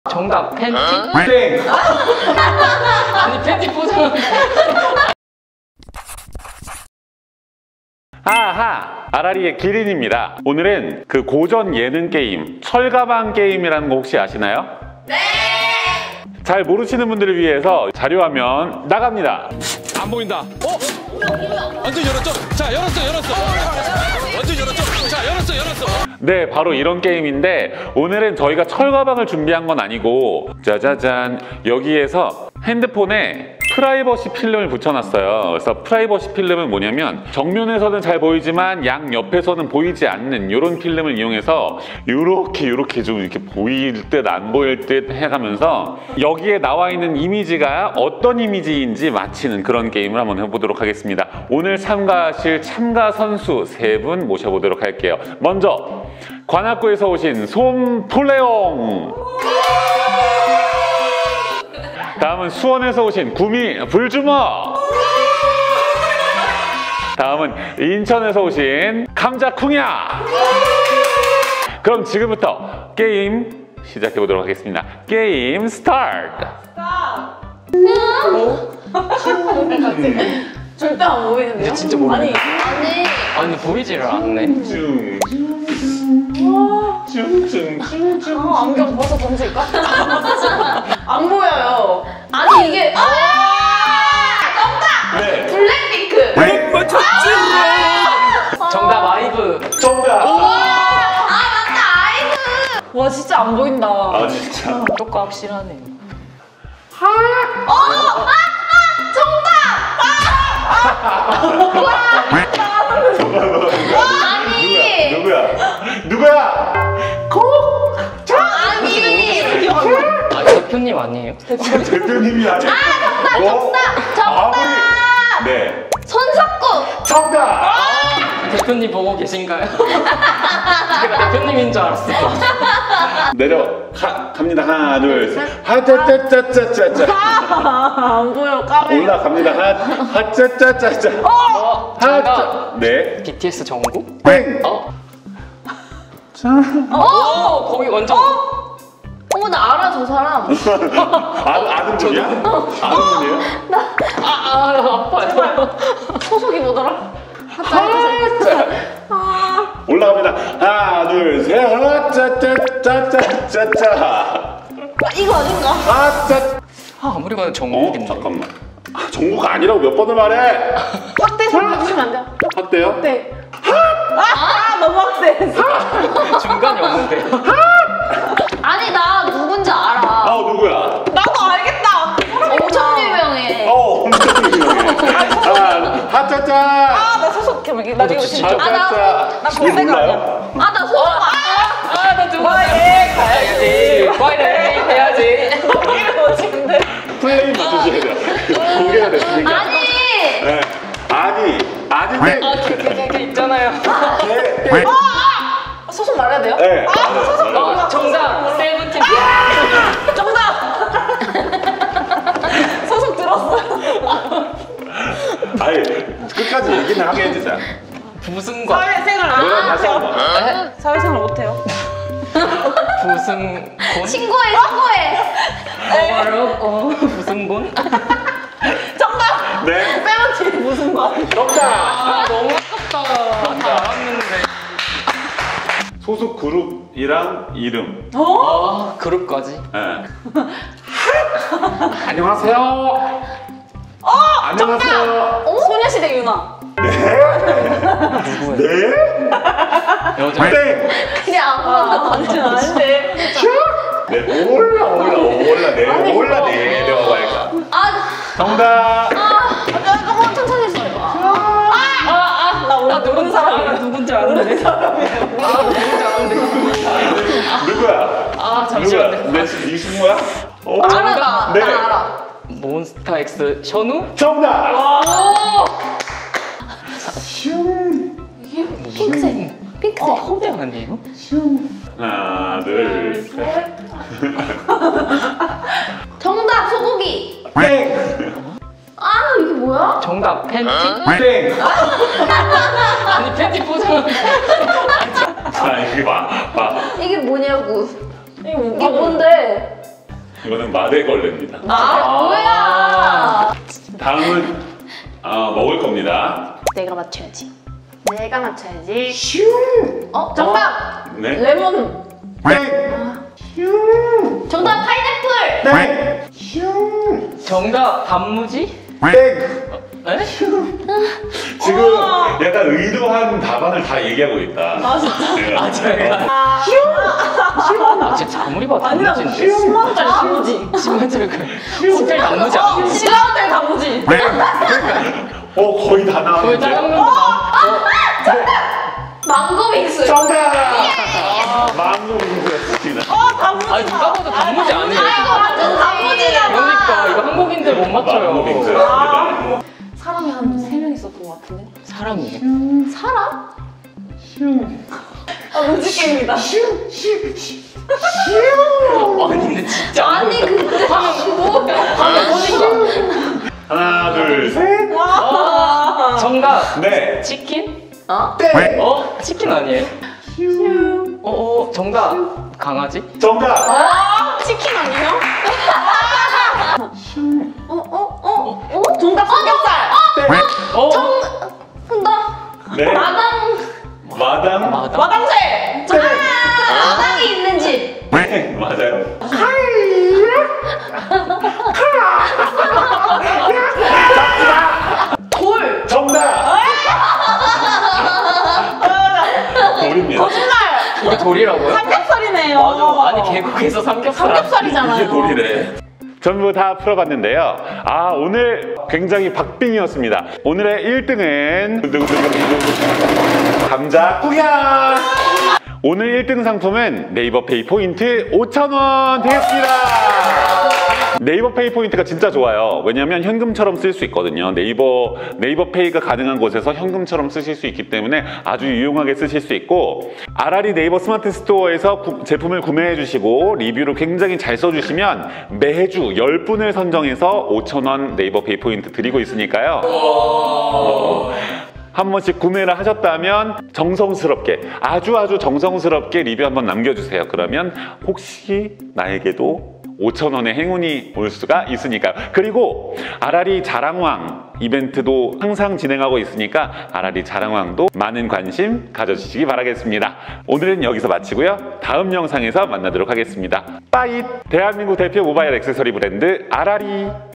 근무시간에 상사랑 게임하기!
안녕하세요 아라리입니다

직장에서 일을 하는것이란... 너무 힘들죠...
그래서 오늘은 저희가 아라리 기린과 함께 일탈(?)을 해보았습니다!

과연! 근무시간에 상사와 게임하는 월루 3인방은 과연 어떻게 되었을까요!?
영상 시청 끝까지~~~ 부탁드릴게요!

아라리 프라이버시 필름!과 강화유리!